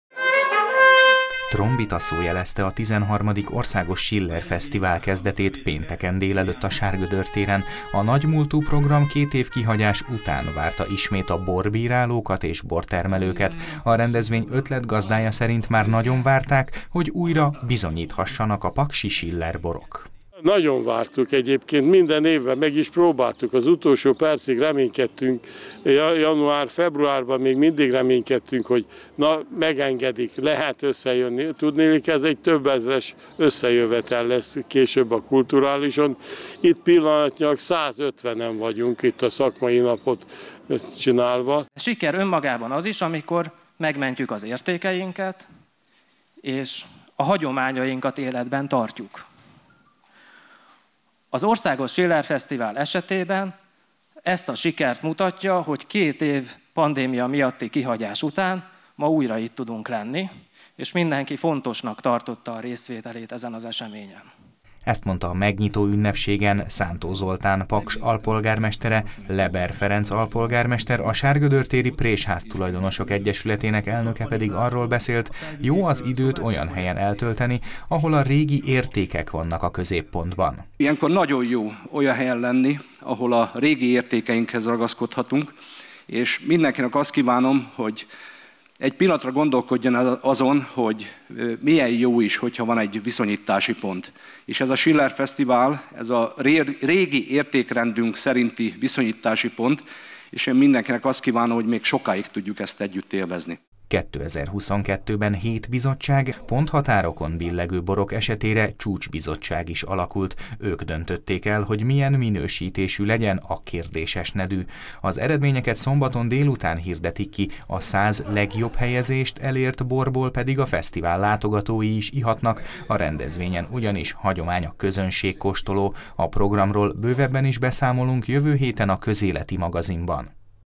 Szombaton kiderül ki készítette a legjobb Sillert
161 mintát vizsgált 7 bírálóbizottság a pénteken délelőtt kezdődött XIII. Országos Siller Fesztivál borversenyén. A bírálat 20 pontos rendszerben zajlott, ponthatárokon billegő borok esetén a csúcsbizottság döntött. A programnak egész hétvégén a Sárgödör tér ad otthont, az eredményhirdetés szombaton lesz, azt pedig közönségkóstoló követi majd.